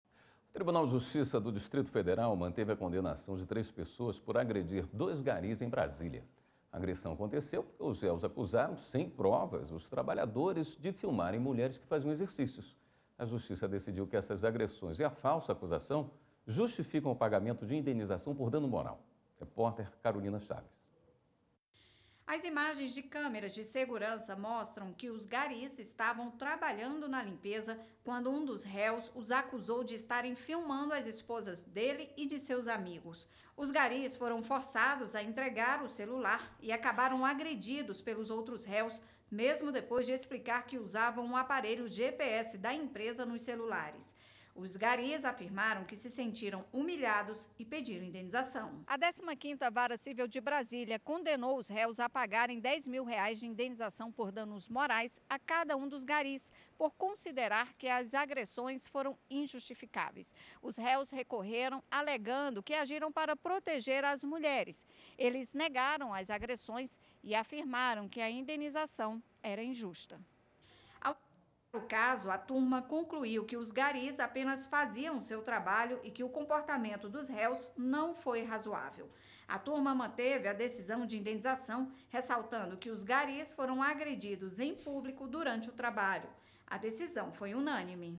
📺 JJ – TJDFT mantém condenação de três pessoas que agrediram garis em Brasília
A agressão aconteceu porque os réus acusaram, sem provas, os trabalhadores de filmarem mulheres que faziam exercícios.